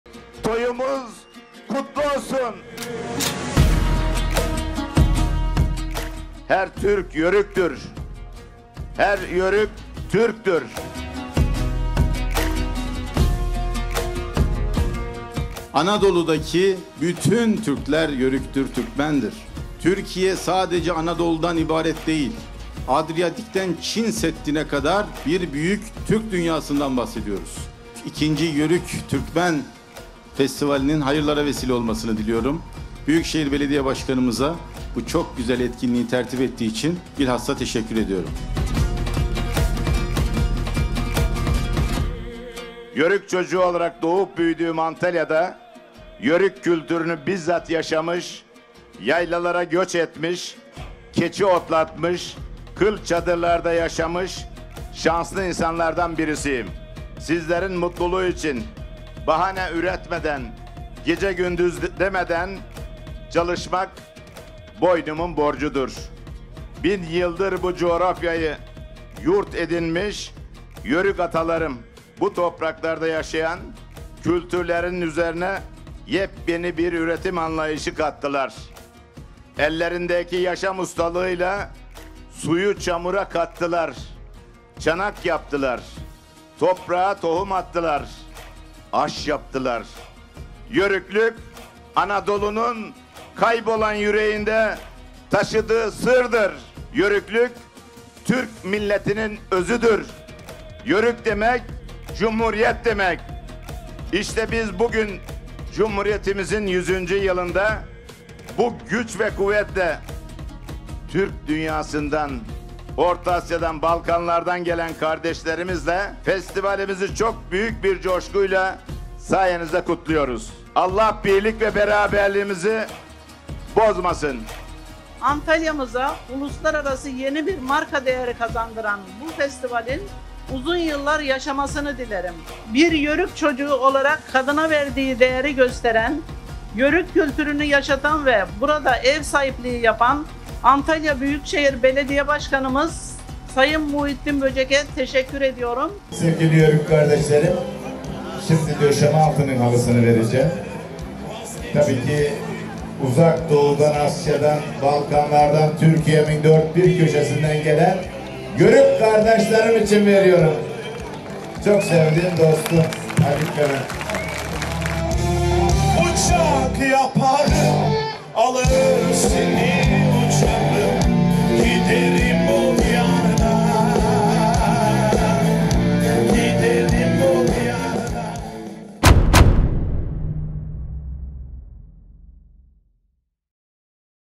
2. Uluslararası Antalya Yörük Türkmen Festivali Türk dünyasını buluşturuyor
Cumhuriyet'imizin 100. Yılı'nda Türk dünyasını Antalya'mızda buluşturmanın gururu ve sevincini yaşıyorum.

2. Uluslararası Antalya Yörük Türkmen Festivali'miz ile Yörük kültürümüzü yaşatıyor ve gelecek nesillere aktarıyoruz. Ne mutlu Türk'üm diyene. 🇹🇷

Toyumuz kutlu olsun.

Muhittin Böcek
ANTALYA BÜYÜKŞEHİR BELEDİYE BAŞKANI

Muhittin Böcek Resmi Youtube Sayfasıdır.
The official Muhittin Böcek YouTube Channel